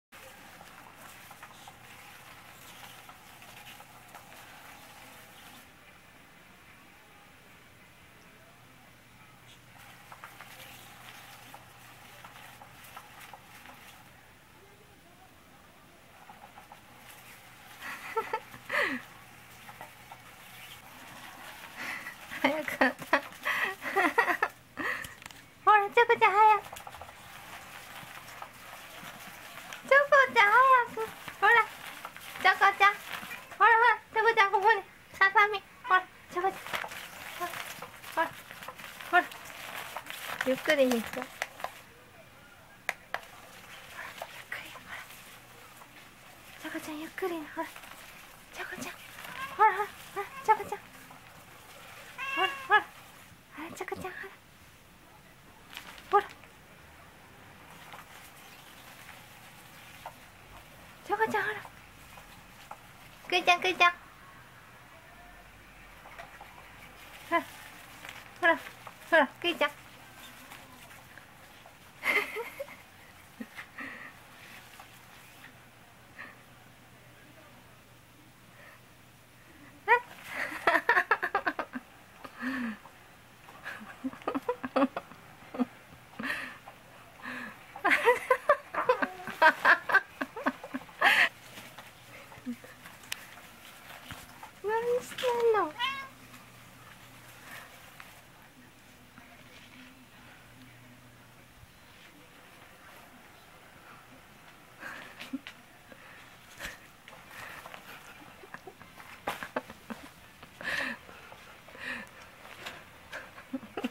めんどくさがりな猫と動くササミに反応する猫
電動玩具キャッチミー２にみんな大好きな乾燥ササミをくっつけてみました。全然遊んでくれないキジトラのチョコは・・・やっぱりな反応(笑)手から貰う時は凄い勢いなんですが(笑)　黒猫のクーは凄いやる気を見せてくれました。
I attached the snacks of the favorite cat to the electric toy and moved it.
The cat which exercise dislikes does not move to think.
Black cat Kuu moved well.